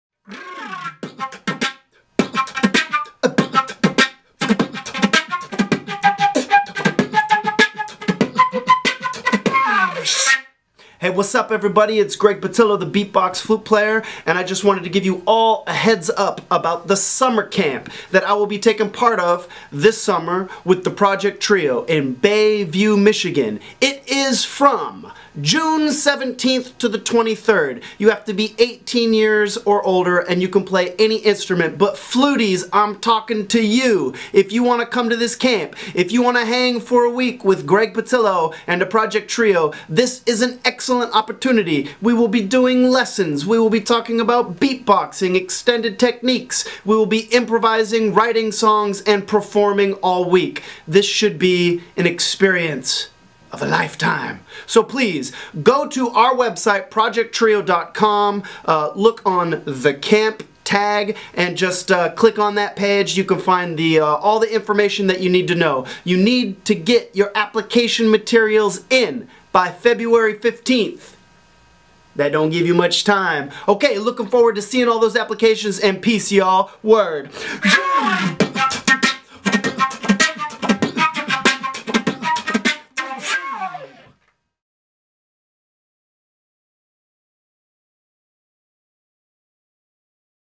PROJECT Trio Summer Camp! - 2012
Get your application materials in by february 15th!  Get your friends to send in an application too!  Any instrument may apply, but you know I'm making a special shout out to all the fluties out there that are trying to get down and bust a move with some beatbox stylings.  just click on the link to The Camp.

NEW ALBUM IS OUT!